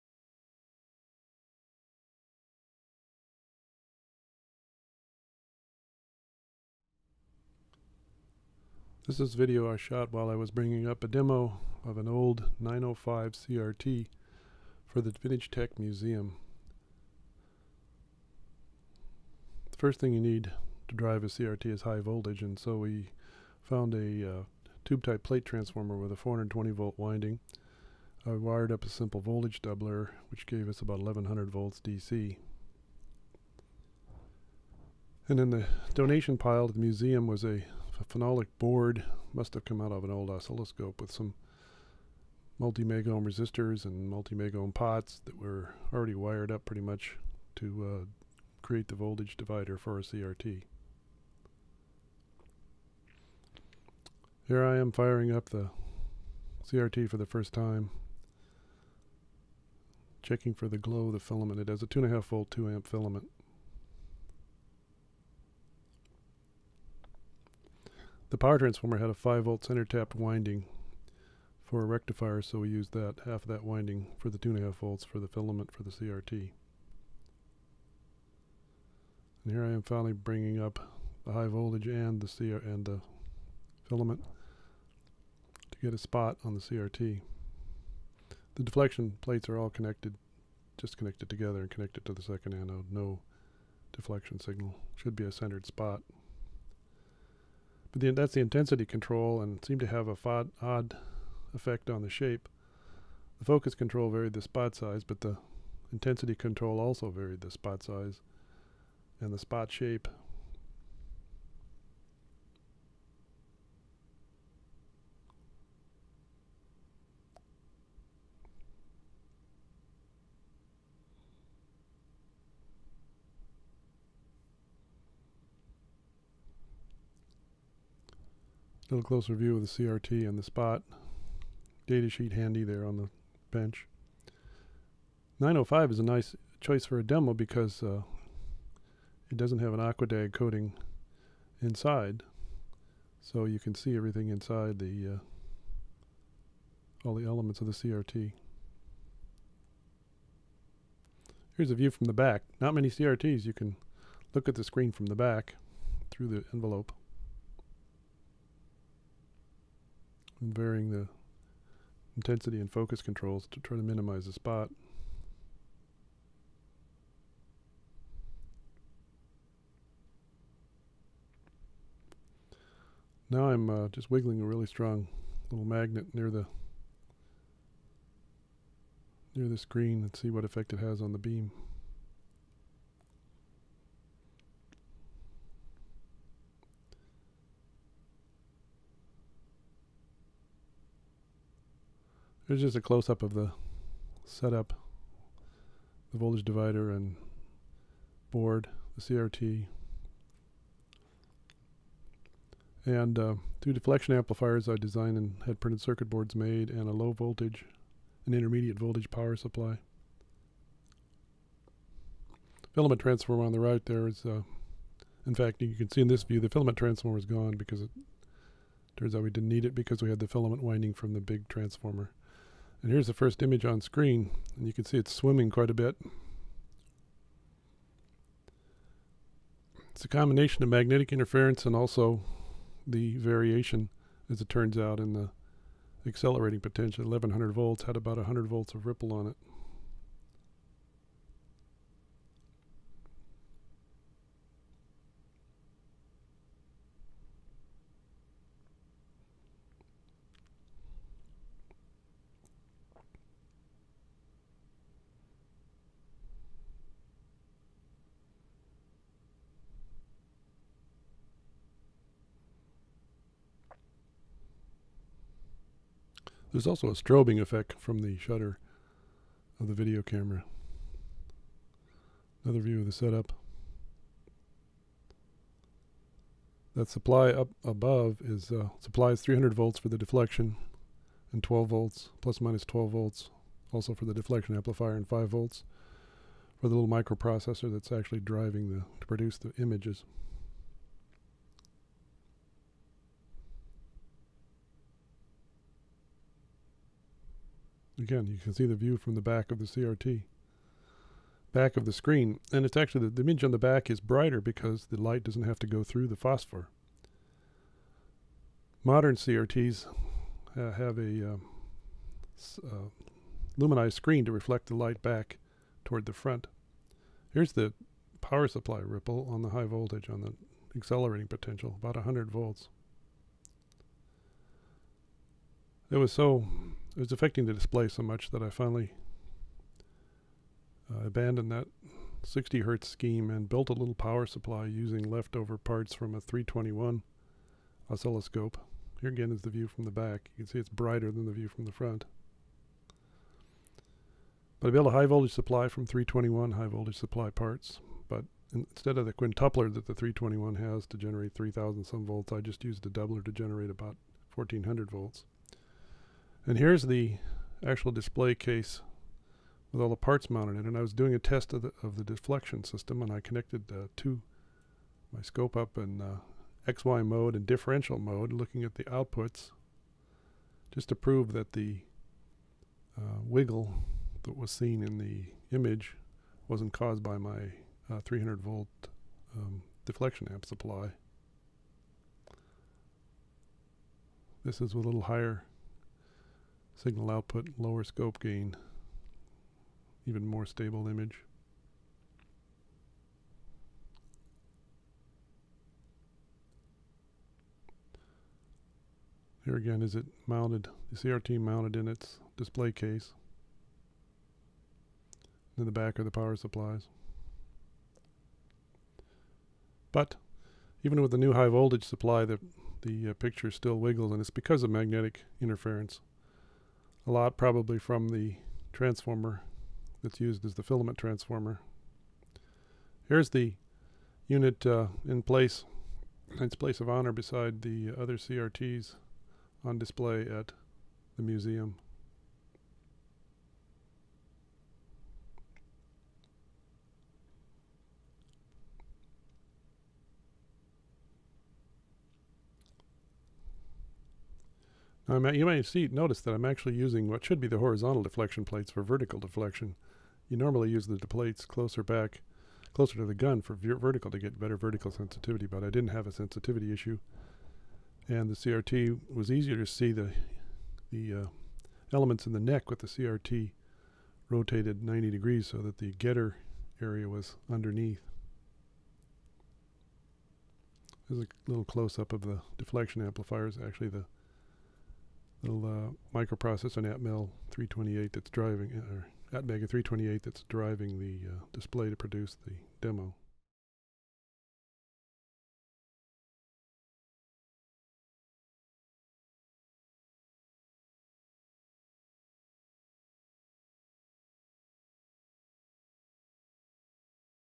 vintageTek Demo of a 1930's 905 CRT
This video documents the creation of a demo for the vintageTek Museum of a 905 CRT (cathode ray tube). This CRT was made by RCA in the late 1930's and is one of the few CRT's made with a clear glass envelope.